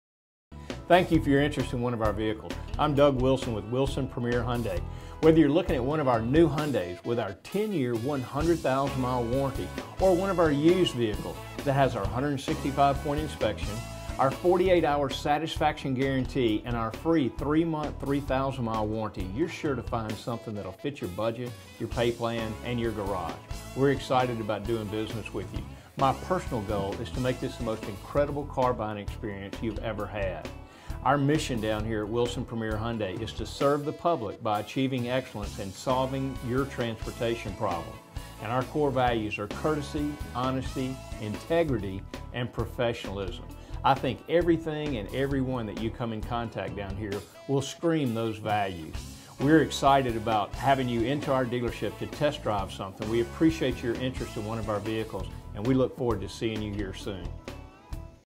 Hello from Doug Wilson at Wilson Premier Hyundai in Ridgeland, MS!
Wilson Premier Hyundai near Jackson, MS. Wilson Premier Hyundai is Mississippi's newest Hyundai dealership located in Ridgeland, MS on I-55 & Steed Road near Jackson, MS. Where there's a Wilson, there's a way!